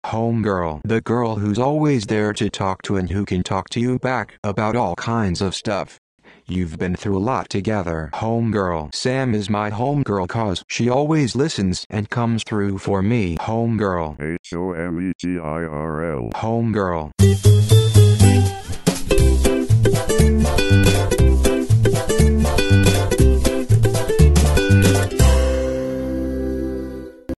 Homegirl meaning and pronunciation
The girl who's always there to talk to and who can talk to you back about all kinds of stuff.  You've been through a lot together.

Homegirl definition by Urban Dictionary